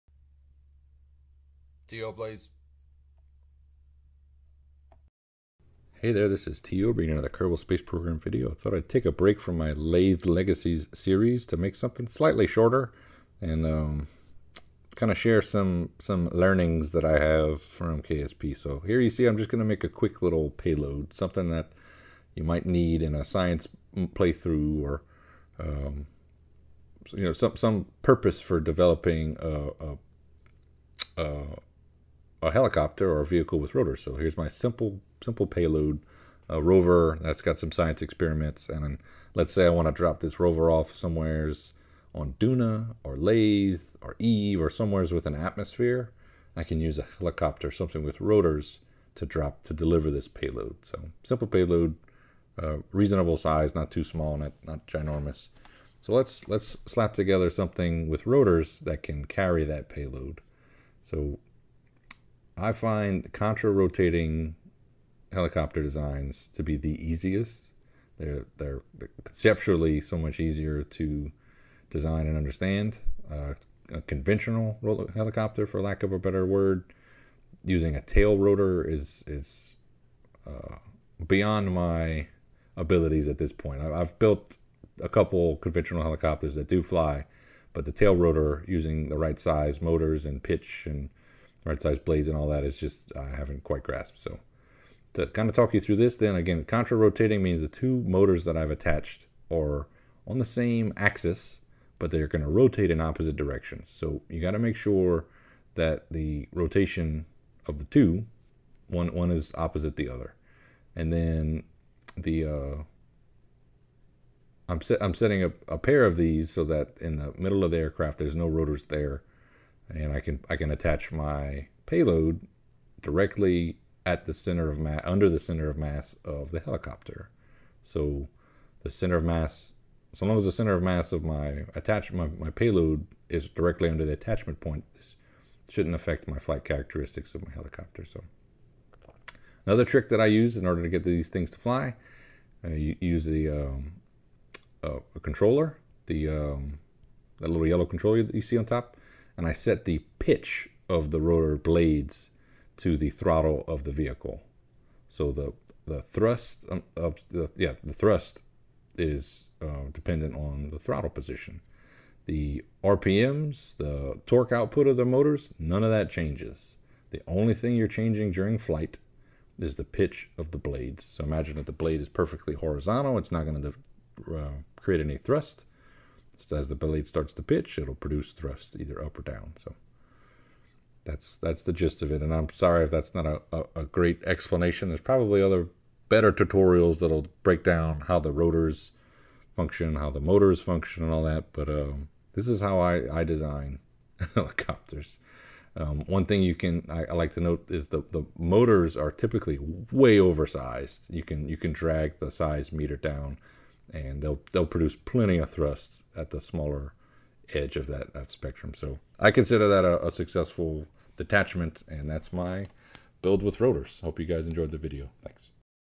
How I Build with Rotors
I give a brief demonstration of how I build things that fly well using rotors.